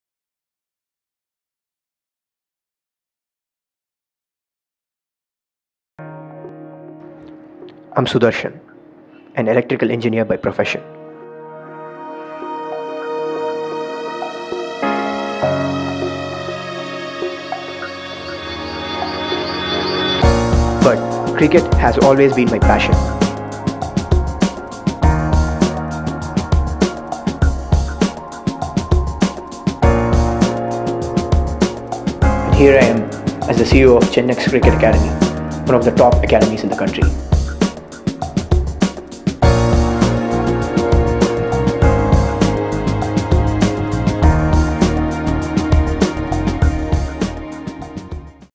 Gen-Next Academy COO Sudarshan's journey from a cubicle to the cricket field | Teaser | Crikvoice
Get a sneak peak of Gen-Next Academy COO Sudarshan's remarkable journey from an IT engineer to becoming a part of one of the best cricket academies in the country mentored by India Test cricketer R Ashwin.